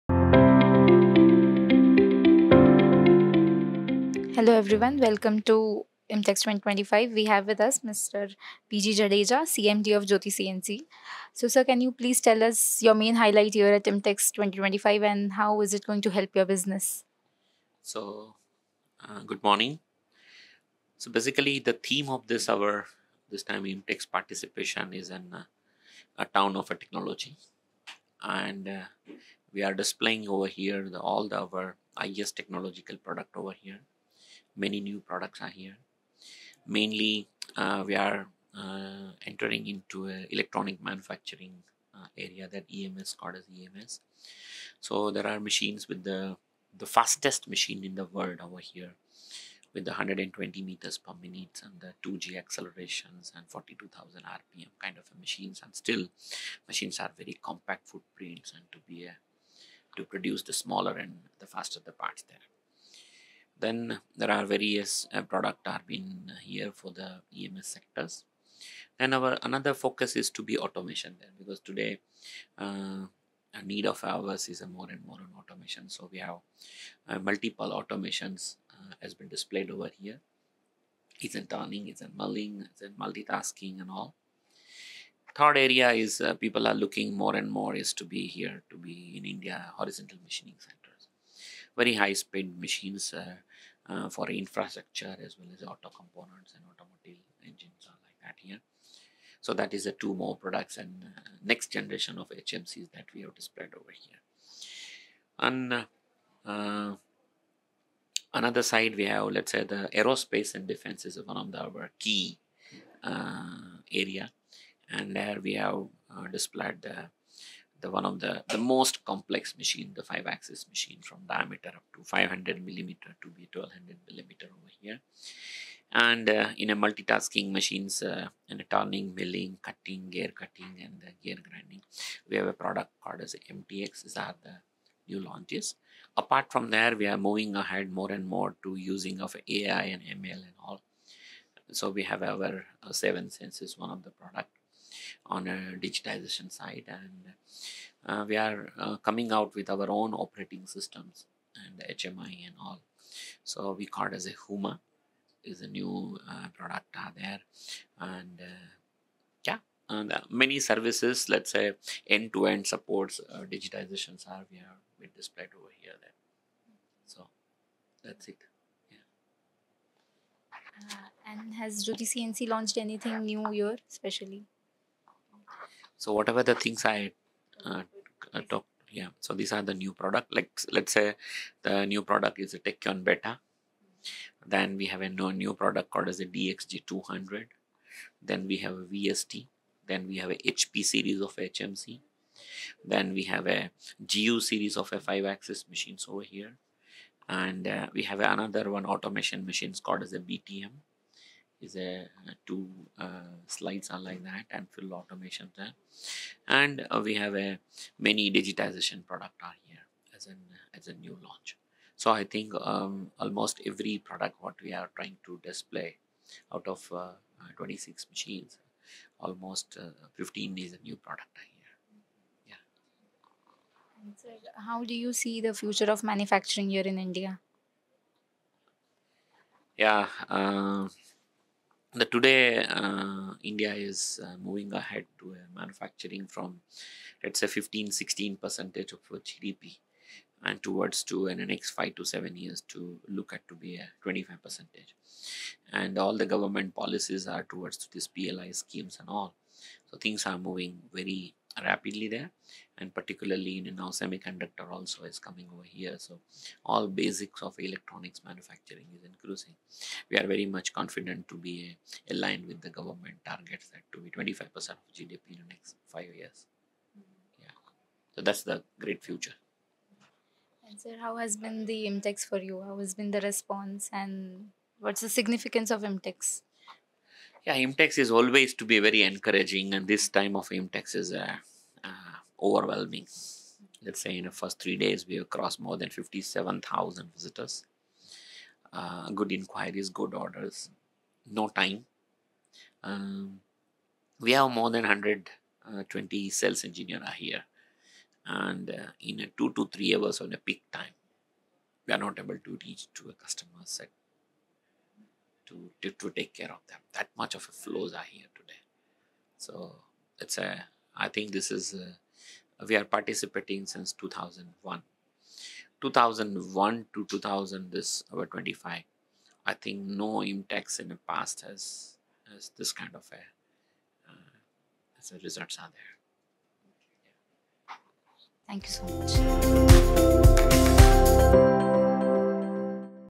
Jyoti CNC presents one of the fastest machines in the world here at IMTEX 2025!
At IMTEX 2025, Jyoti CNC Automation Limited made a powerful statement with its groundbreaking innovations and expansion into electronics manufacturing. Speaking about their focus areas, P.G. Jadeja, CMD, Jyoti CNC, highlighted the company’s vision for the future, emphasizing their role in shaping India's manufacturing landscape.

A major highlight was the unveiling of one of the fastest machines in the world, showcased at their massive “Town of Technology”—a stall that left visitors in awe. The presence of Jyoti CNC was unmistakable, reflecting their dominance and commitment to advanced manufacturing.
Automation was another key focus, with multiple cutting-edge machines on display, alongside horizontal machining solutions. Additionally, aerospace and defense remained a priority, featuring complex 5-axis machines engineered for precision and efficiency.